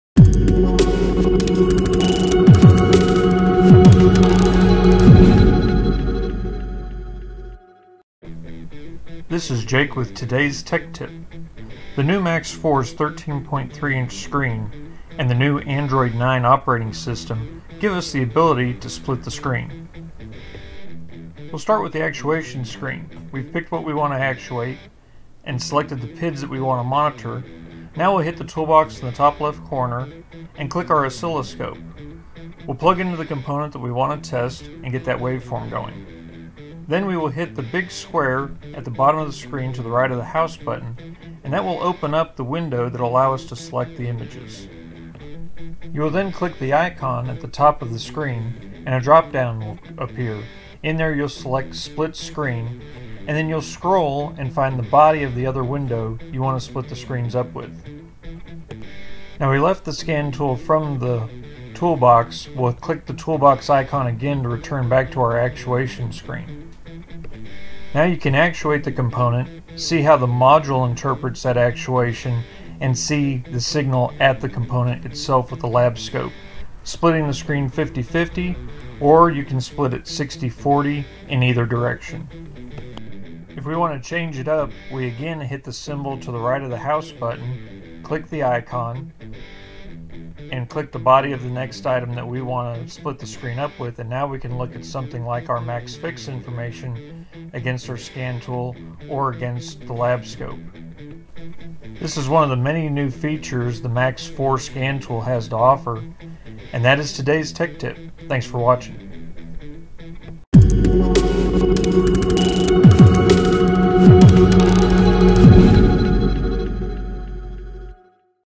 Tech Tip: How to split the screen of your Max4 Scan Tool
This tutorial shows you how to quickly split the screen of your Maximus 4.0 Scanner (MDMAX4CL, MDMAX4HD, MDMAX4CLHD)